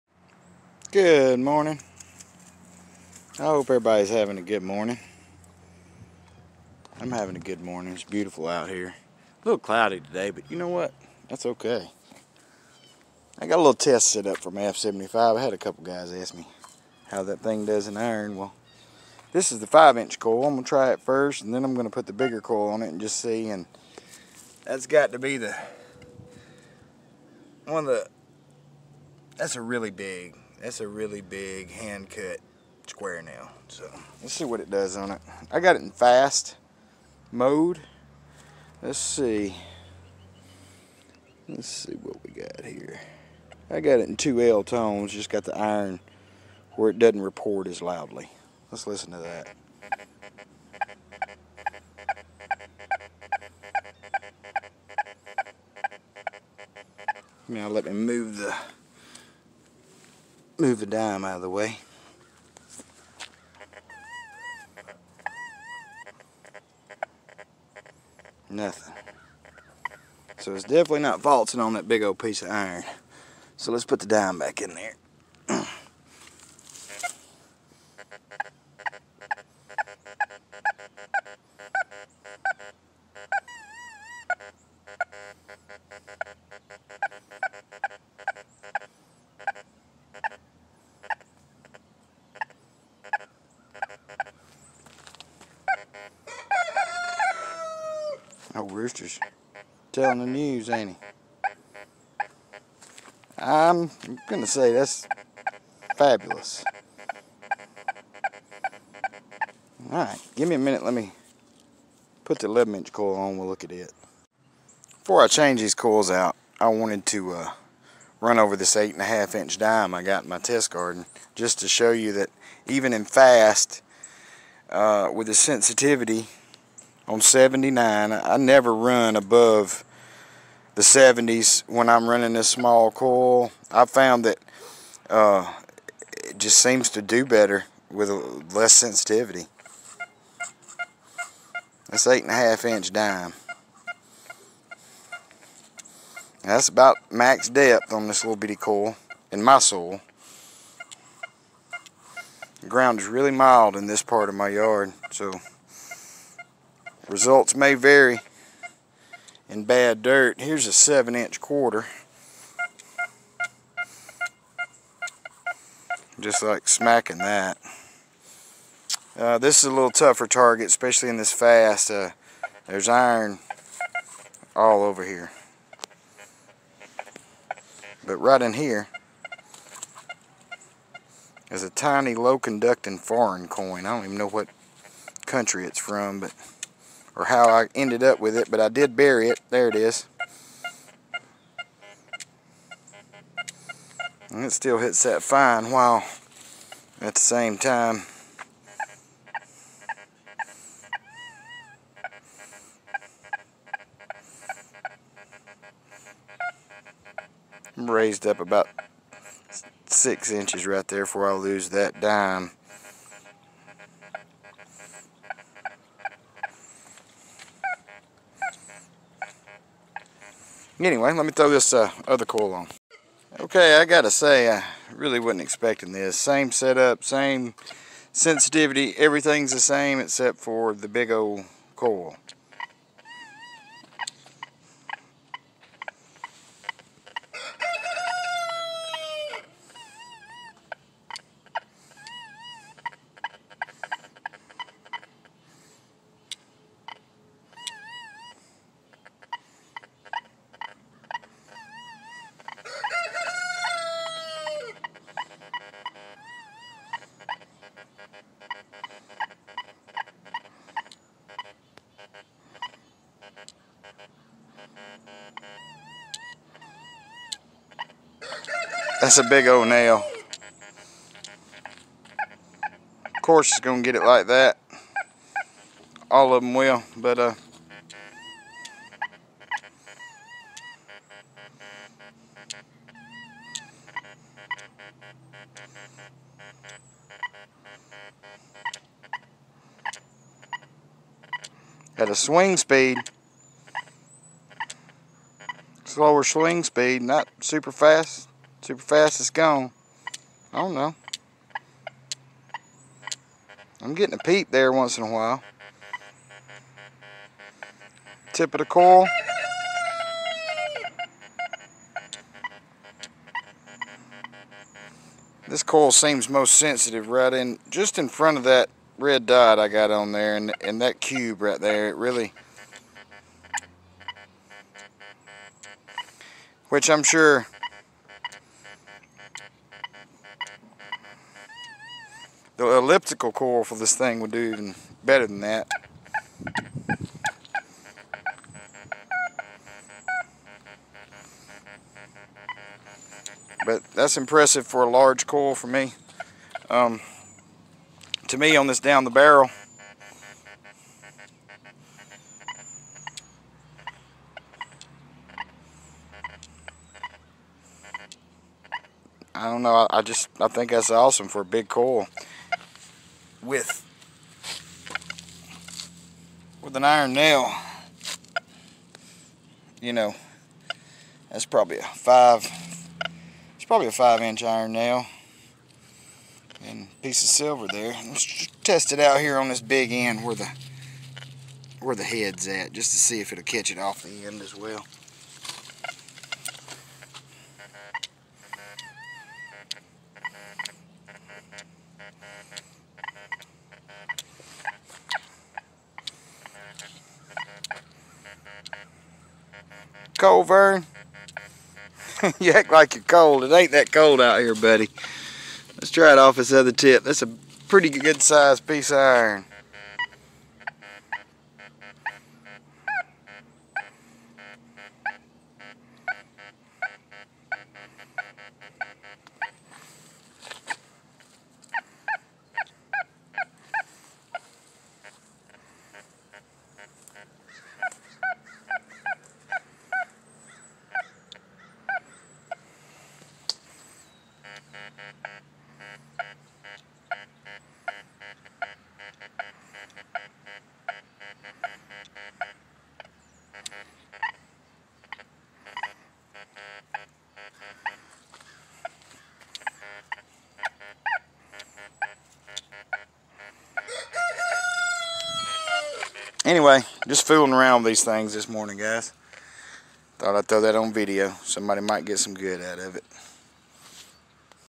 Fisher F75se vs large hand cut nail test.
F75se vs large iron nail stock coils.
Needed a base line test. Gonna try the NEL Superfly soon.